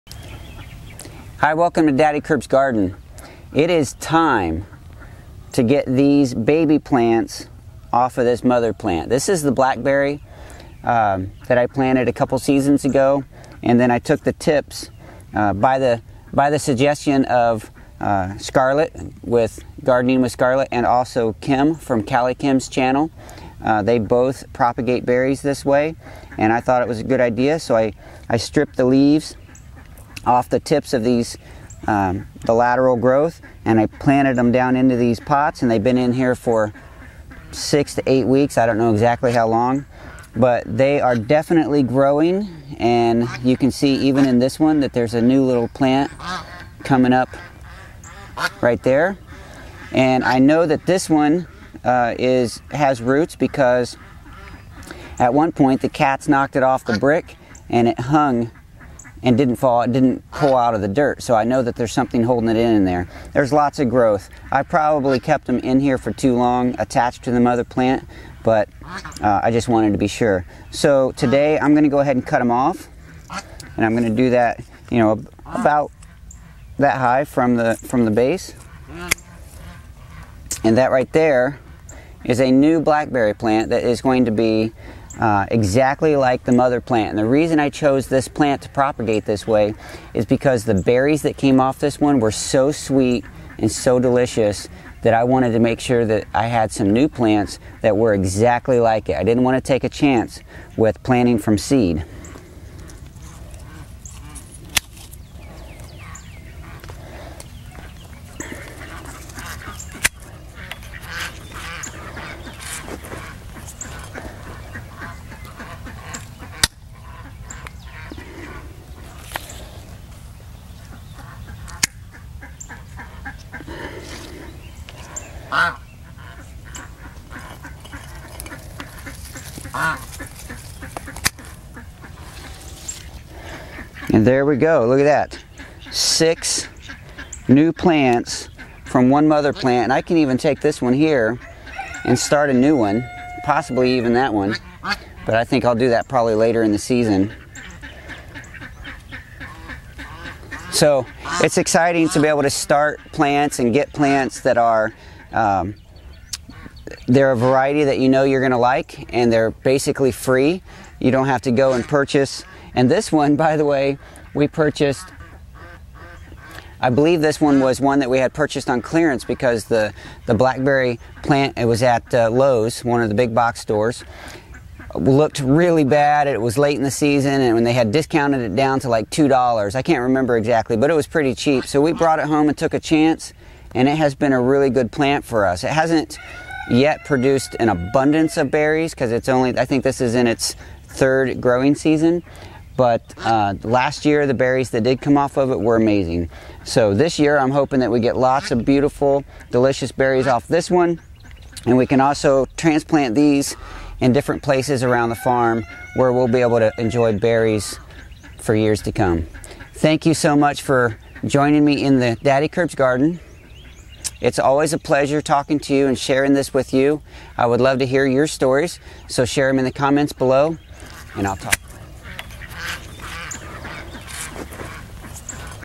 How I Got 6 FREE Blackberry Plants
Scarlett and Kim both have videos on how to get free berry plants. I paid attention so my little farm could benefit from all these free plants just like their gardens. These next few minutes can show you how to save money in your garden too.